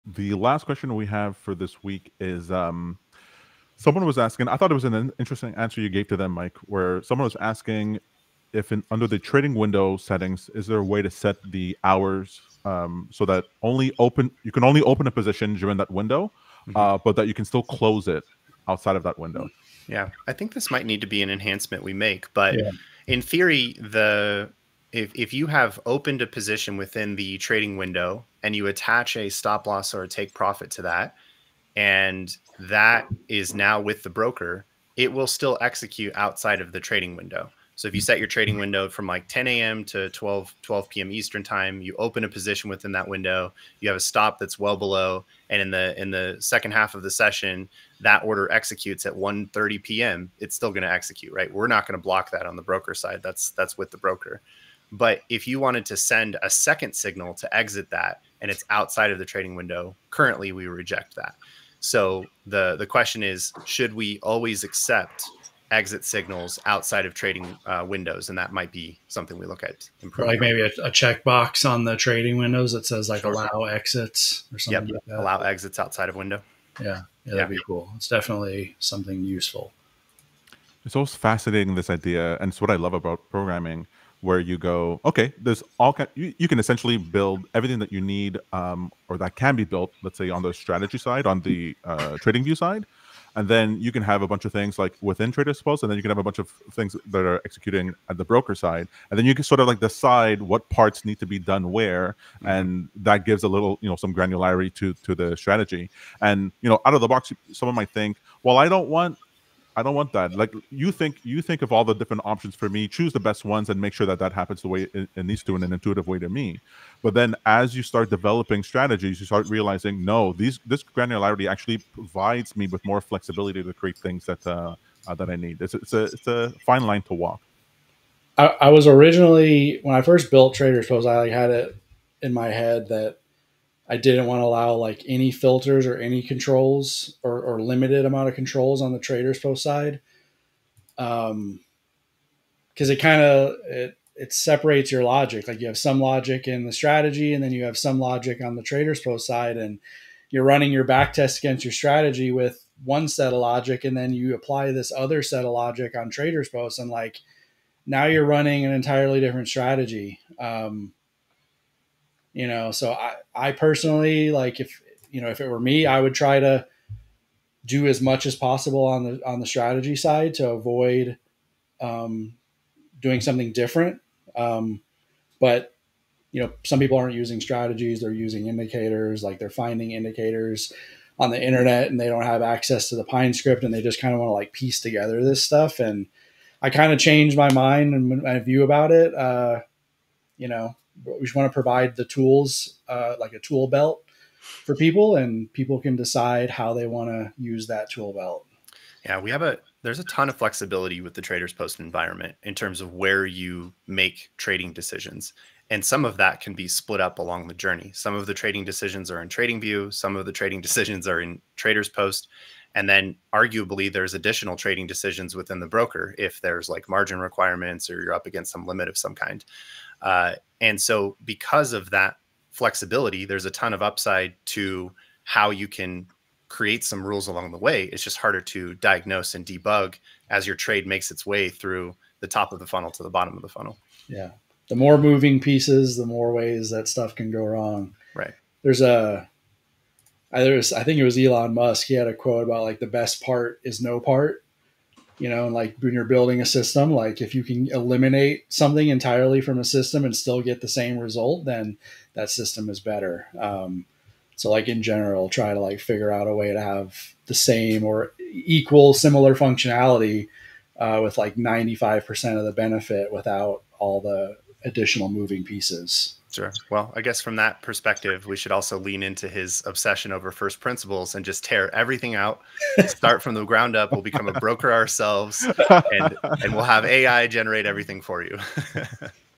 Can I close my position outside of my TRADING WINDOW?
When automating trading strategies, bracket orders allow exit orders to be executed outside of the trading window setting.